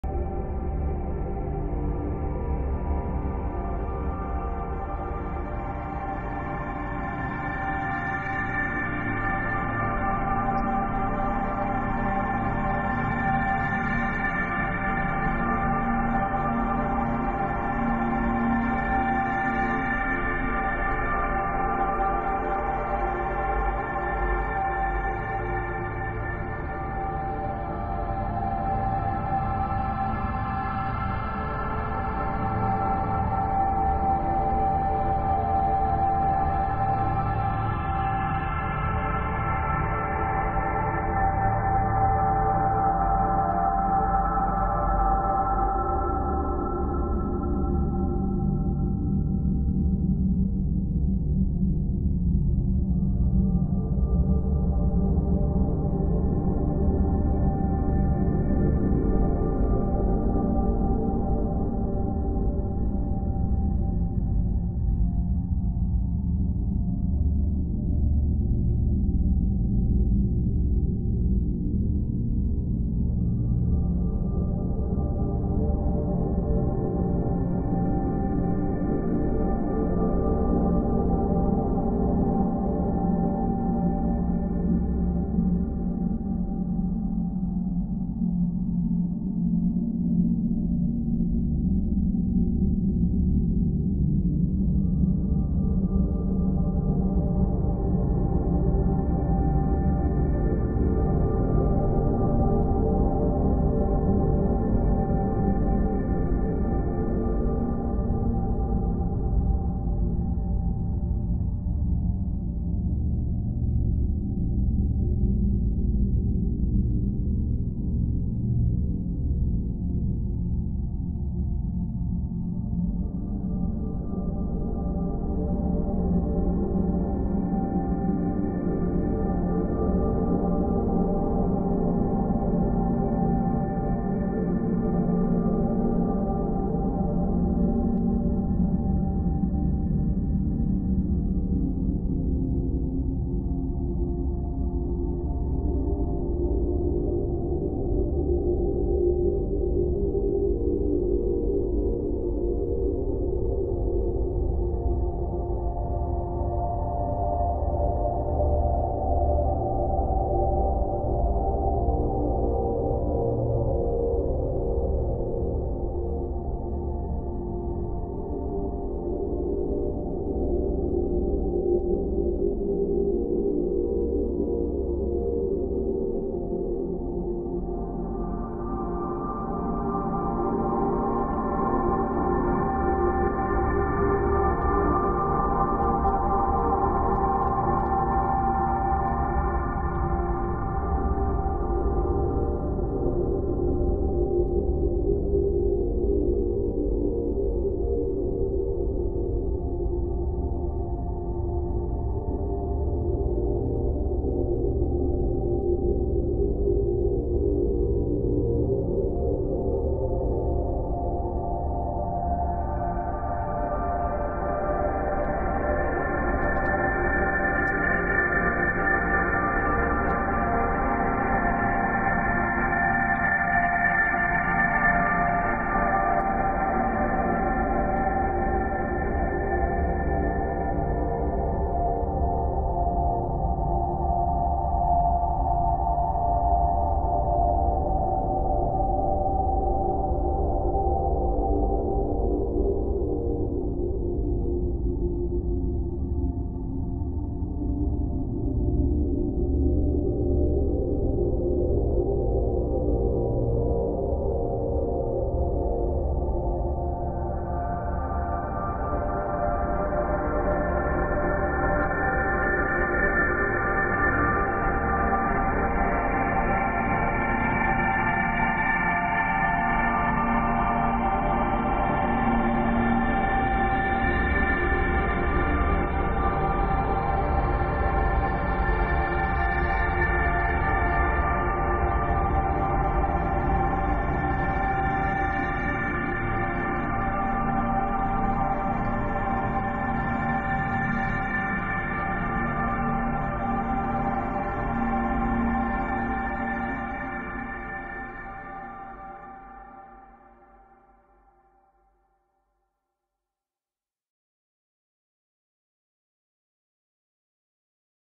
Listen to this to attract an abundance of money !!
Attract money your way by listening to this five minute video